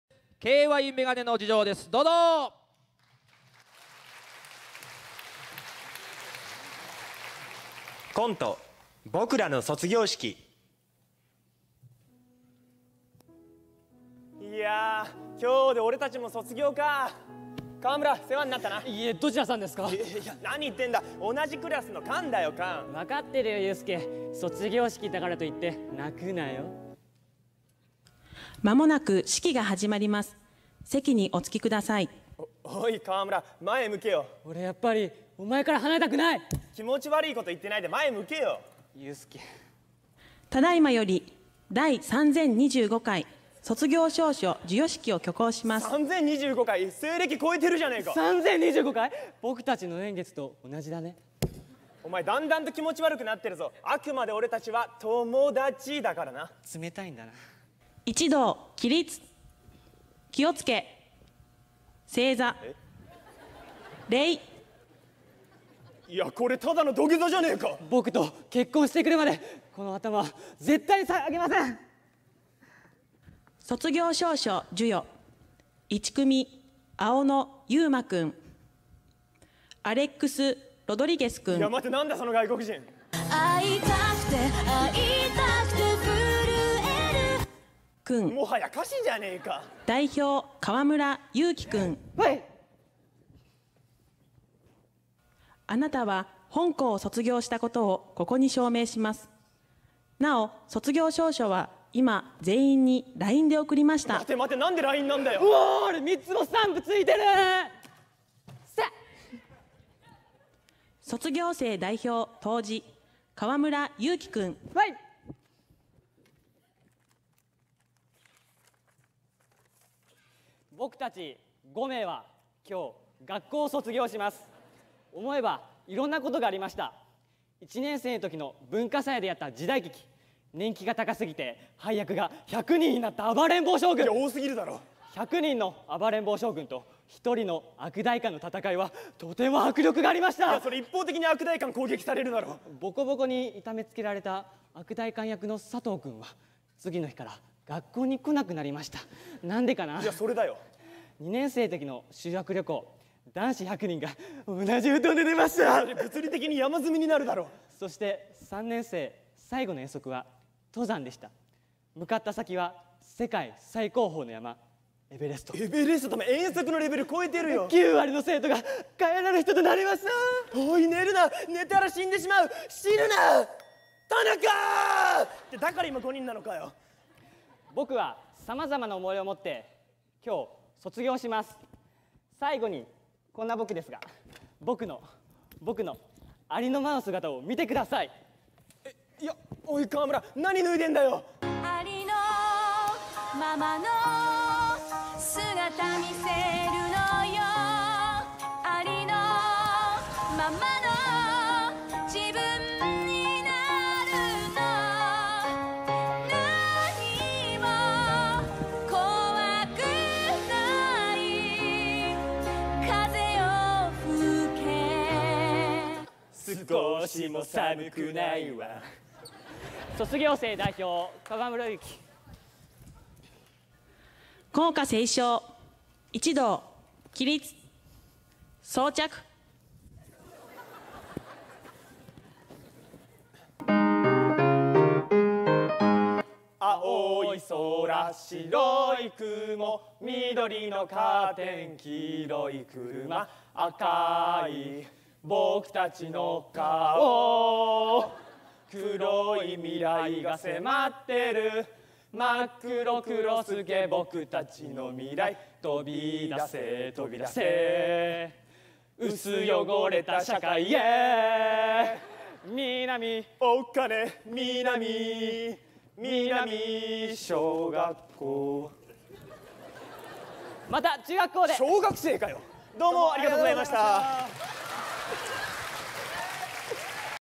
第4回笑顔甲子園 KYメガネの2乗 決勝バトル
８月２４日（日）市民文化センター中ホールで行われた、第４回笑顔甲子園決勝バトルのうち、　新居浜市のKYメガネの2乗の漫­­才です。